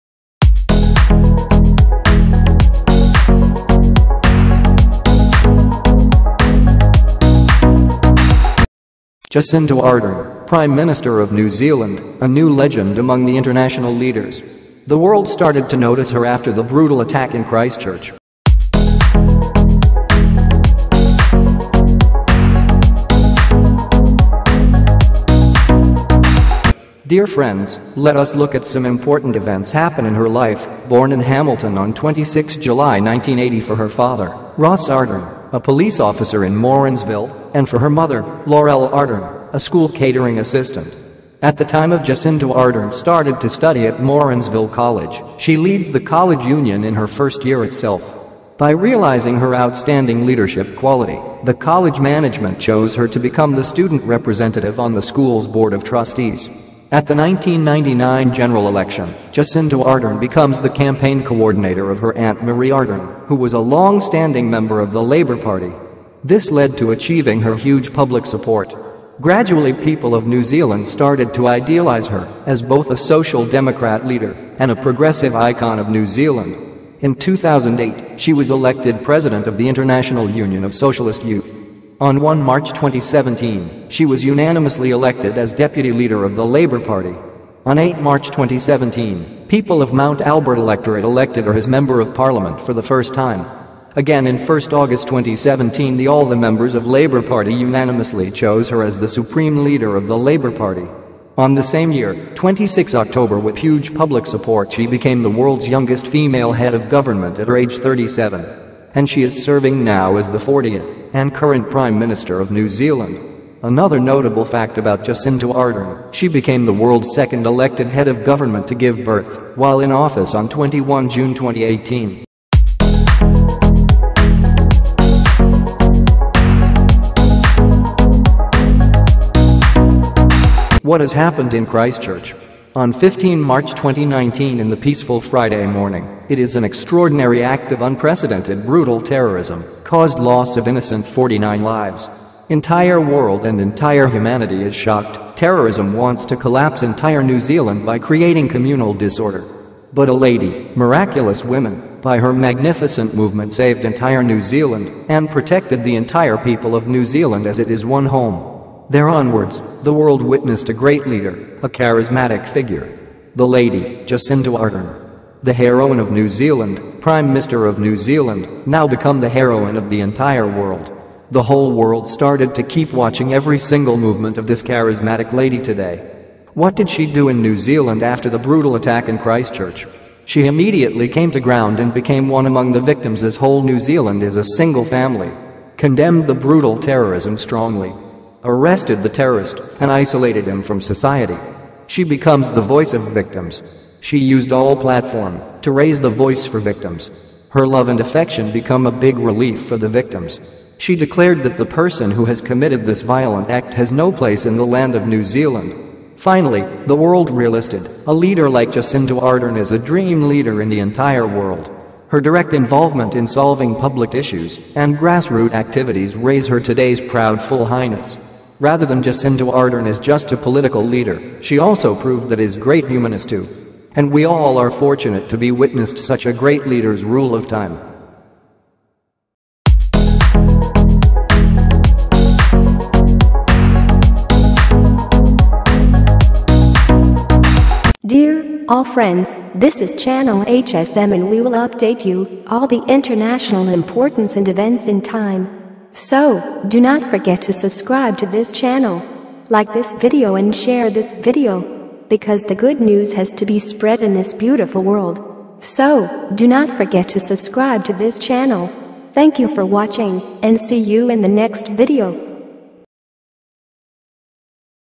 Jacinda Ardern: A leader of surprise, New Zealand Prime minister
Prime minister of New Zealand: Jacinda Ardern.
Born in Hamilton on 26 July 1980 for her father, Ross Ardern, a police officer in Morrinsville, and for her mother, Laurell Ardern, a school catering assistant.
At the time of Jacinda Ardern started to study at Morrinsville College,
She leads the college union in her first year itself.
By realizing her outstanding leadership quality, the college management chose her to become the student representative on the school's Board of Trustees.